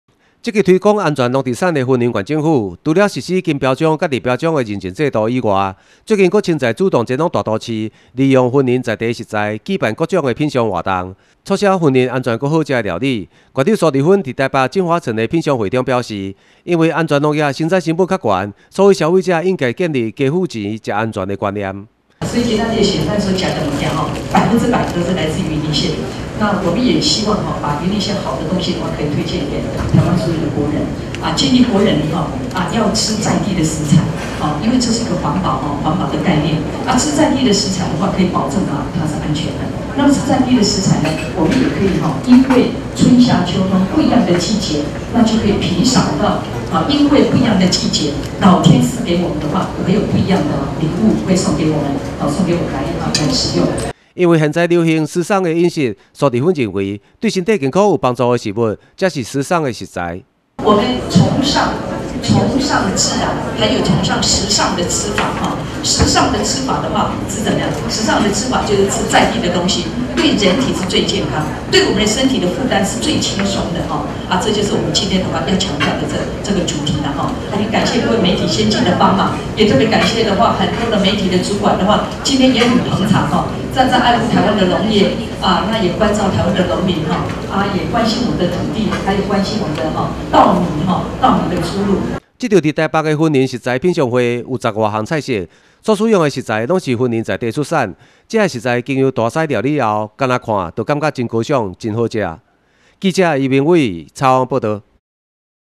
雲林新聞網─斗六縣府推廣時尚農產
雲林縣政府為呼籲全國民眾，購買安全優質的農特產品，特別在台北京華城餐廳，舉辦一場雲林在地食材的品嚐活動；縣長蘇治芬強調，時尚的食物應該是對身體健康有好處的食材，現在消費者也應該建立要吃安全的食物，就應該多付費的觀念，這種觀念對從事安全農業的農友來講，是一個很大的支持力量。【以上新聞可從哈TV App收視】